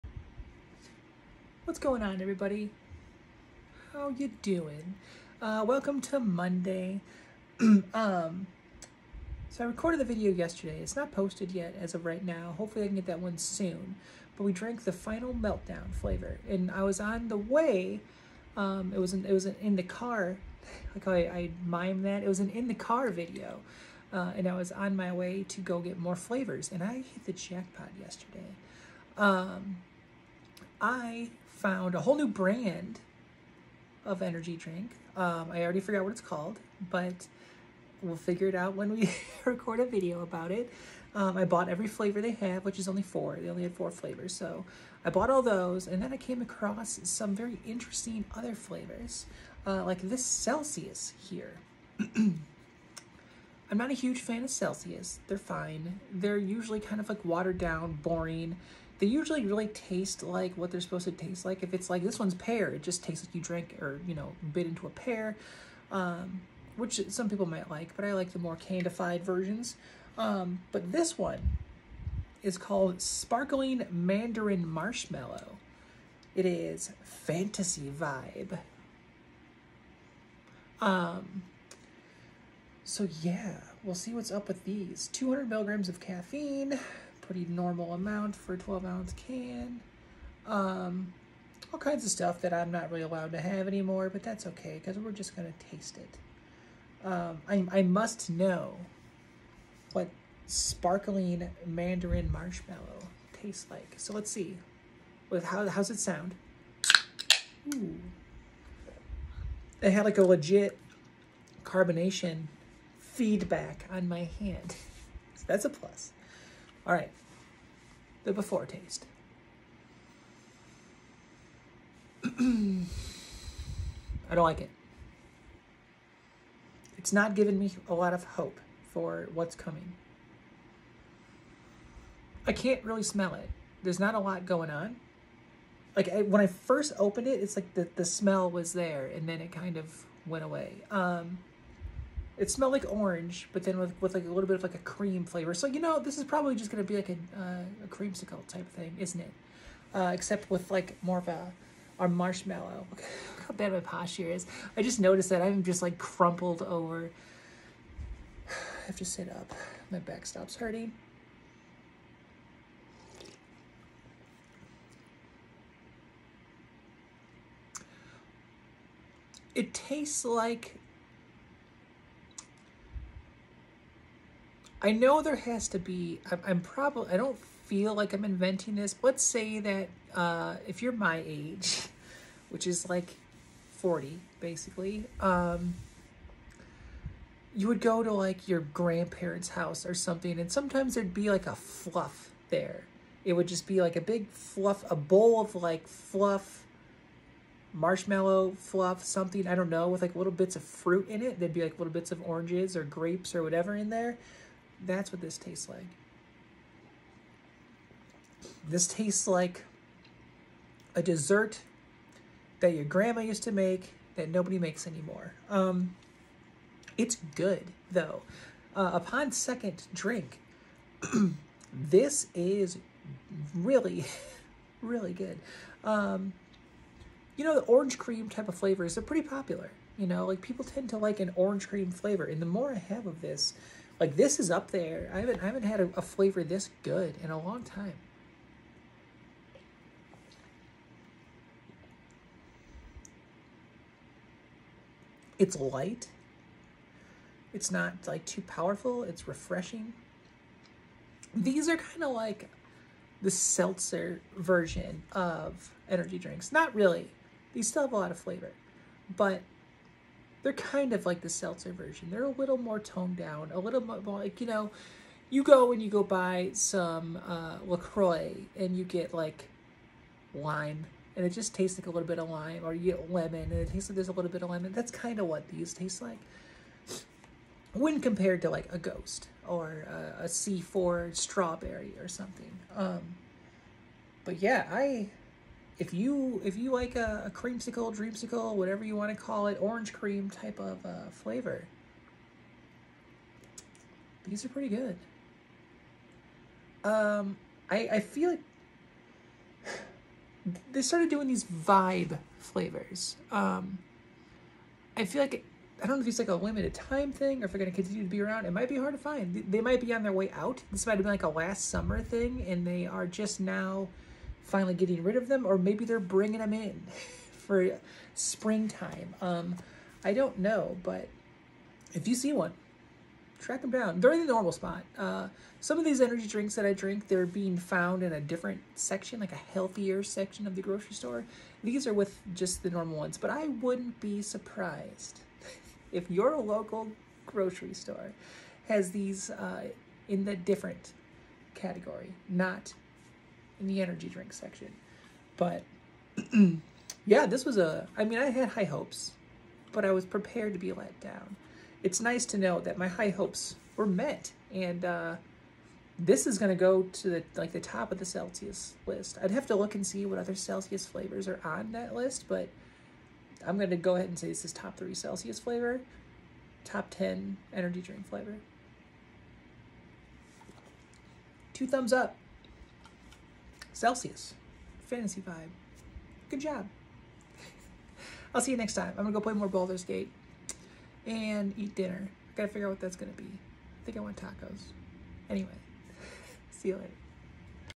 Zenergi Drinks: Celsius sparkling mandarin marshmallow
New flavor alert! Super excited to try this one. Marshmallow??